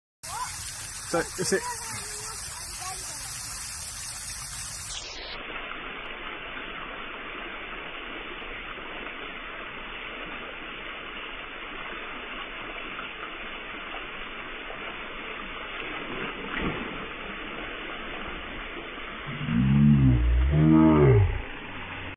Girl Falls In A Fountain While Posing For Photos | Funny Instagram Fail
Funny Falling Video
Falls In Fountain
Woman Falling In Fountain